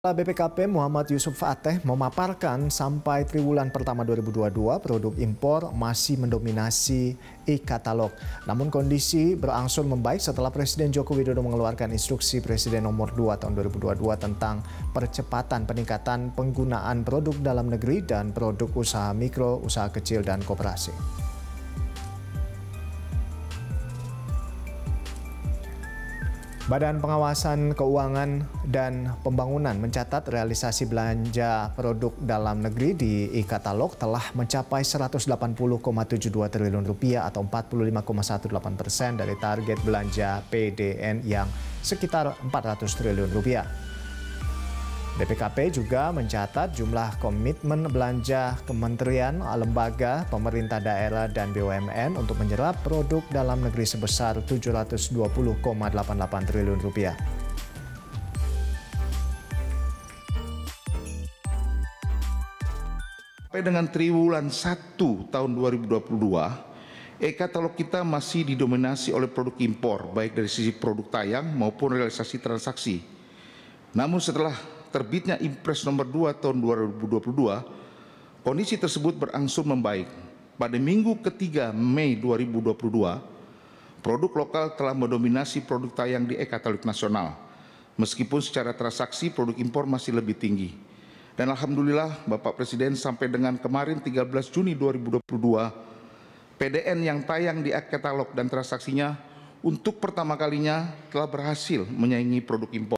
Produk Impor Masih Dominasi E-Katalog Hingga Triwulan Pertama 2022
Kepala BPKP Muhammad Yusuf Ateh memaparkan sampai triwulan pertama 2022, produk impor masih mendominasi e-katalog. Namun kondisi berangsur membaik setelah Presiden Jokowi mengeluarkan inpres nomor 2 tahun 2022 tentang percepatan peningkatan penggunaan produk dalam negeri dan produk usaha mikro, usaha kecil, dan koperasi.